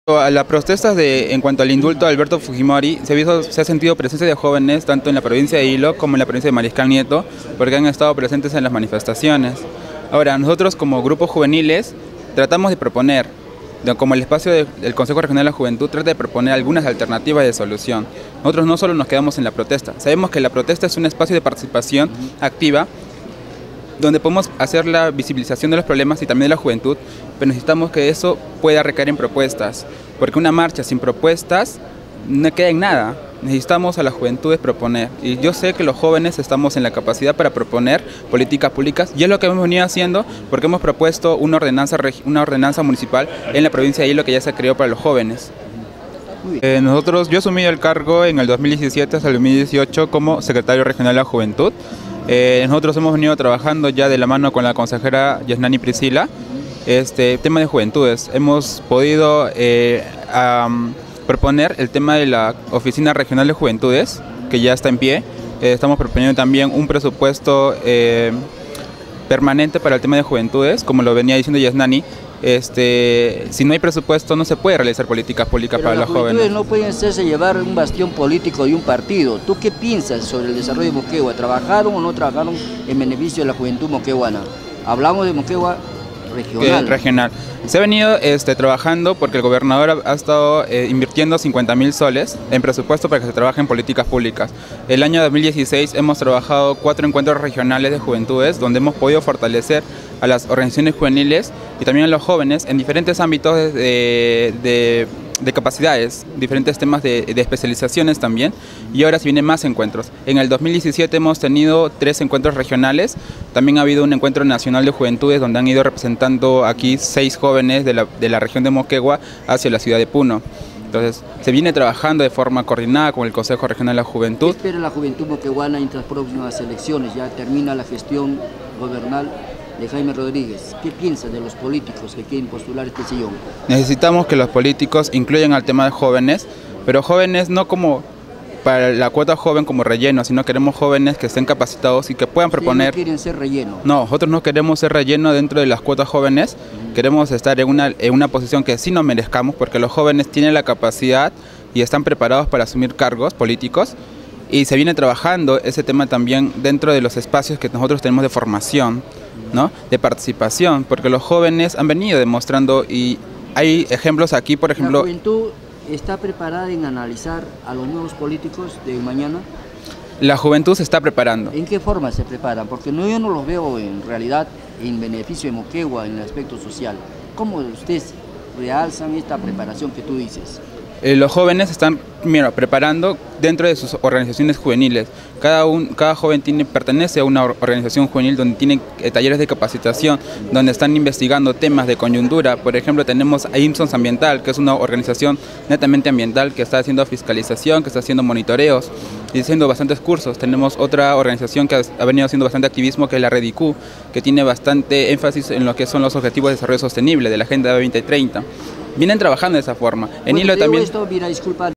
SECRETARIO REGIONAL DE LA JUVENTUD MOQUEGUA.04/01/18
El Joven Leo Dan Apaza, Secrertario Regional del COREJU, manifestó que trabaja5rá a mano con la nueva consejera delegada Yesnani Pricilia Quico que el día de hoy, juramentó como consejera Delgada para el periodo 2018.
Así mismo habló sobre los trabajos que realiza como secretario de la COREJU en la región Moquegua.